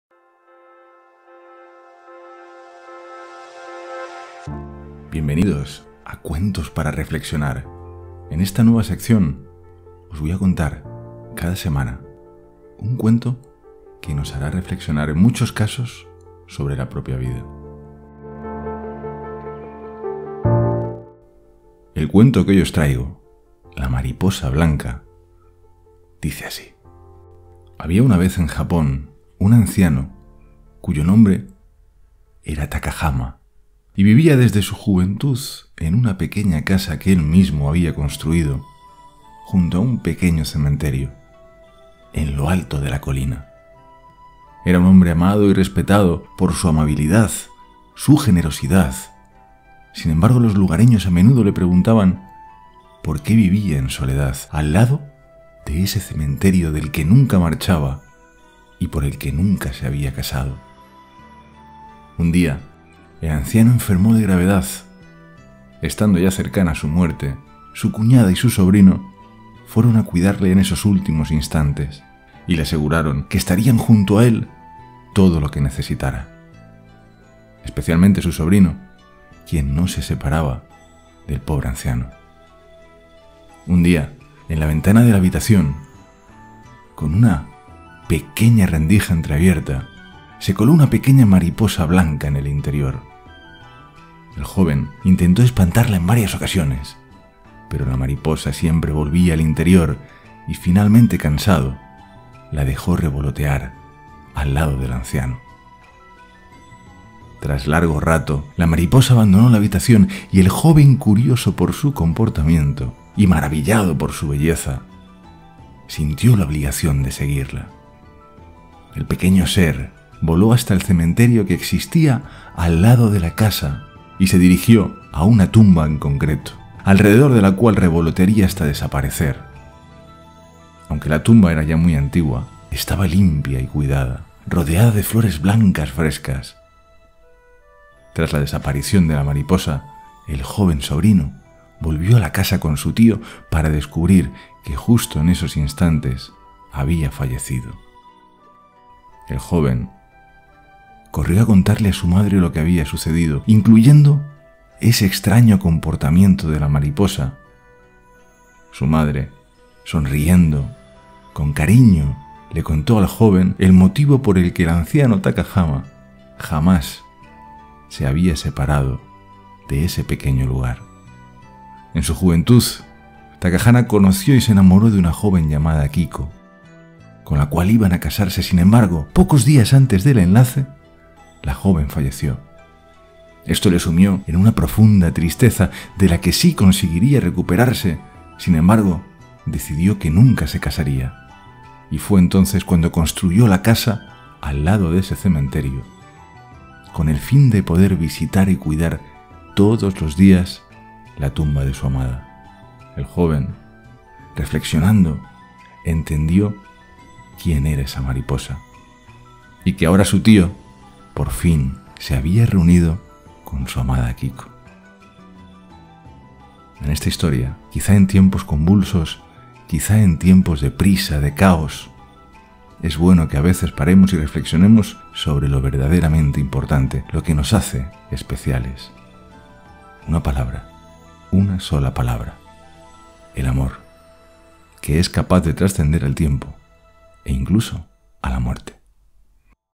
La Leyenda de la Mariposa Blanca / El Cuenta Cuentos / Historias para Reflexionar
“Había una vez en Japón un anciano cuyo nombre era el de Takahama, y que vivía desde su juventud en una pequeña casa que él mismo había construido junto a un cementerio, en lo alto de una colina. Era un hombre amado y respetado por su amabilidad y generosidad, pero los lugareños a menudo se preguntaban porqué vivía en soledad al lado del cementerio y por qué nunca se había casado.
¿Quieres saber el resto?

✔ Entra YA
✔ Activa la 🔔